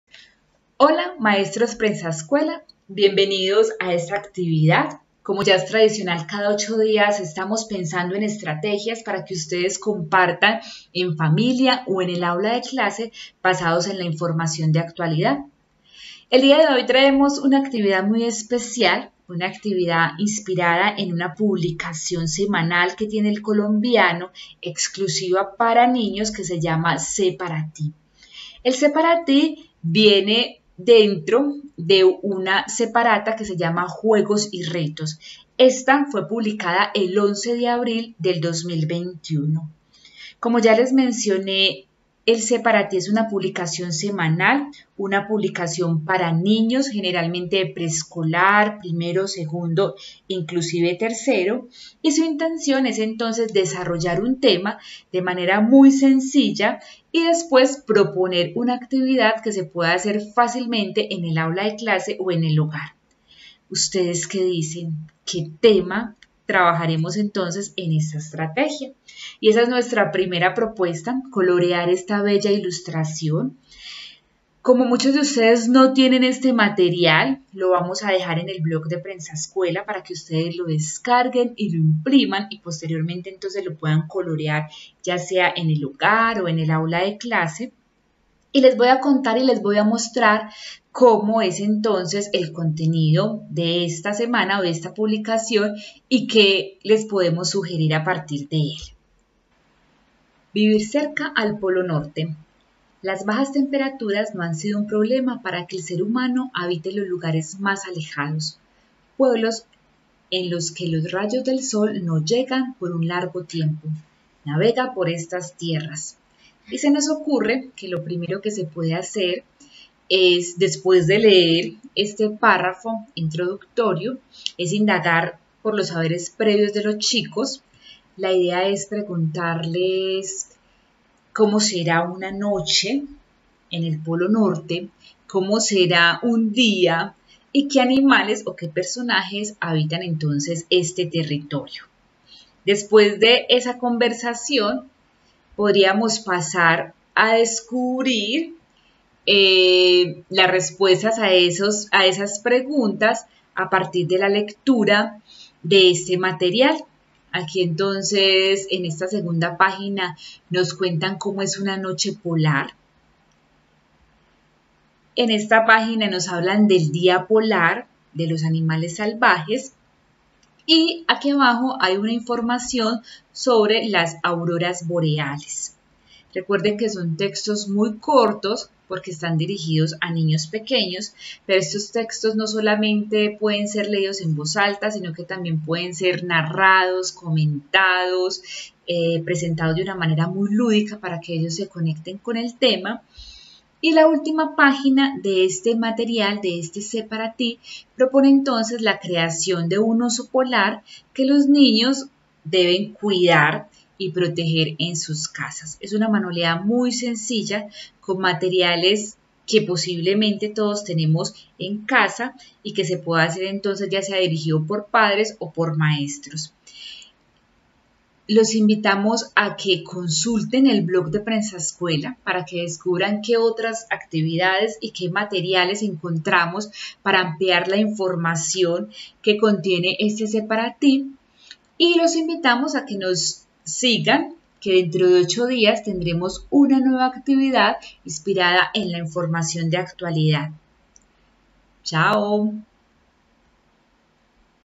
Vivir cerca del Polo Norte
Las bajas temperaturas no han sido un problema para que el ser humano habite los lugares más alejados, pueblos en los que los rayos del sol no llegan por un largo tiempo.

La posición de este territorio en el globo terráqueo hace que sus estaciones sean muy diferentes a las que se viven en el resto del mundo, por lo que sus habitantes presencian fenómenos únicos.